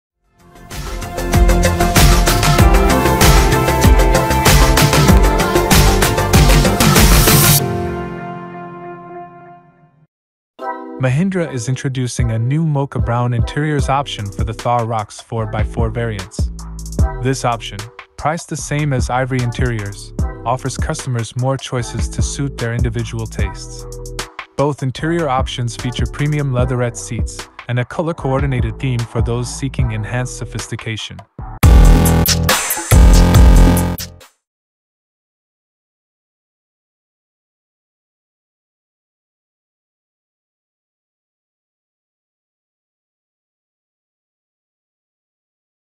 Mahindra Thar Roxx 4x4 Mocha Brown interior images released | AUTOBICS
Mahindra is introducing a new Mocha Brown interiors option for the Thar Roxx 4x4 variants. This option, priced the same as Ivory interiors, offers customers more choices to suit their individual tastes. Both interior options feature premium leatherette seats and a color-coordinated theme for those seeking enhanced sophistication.

Great Discounts on Car & Bike Accessories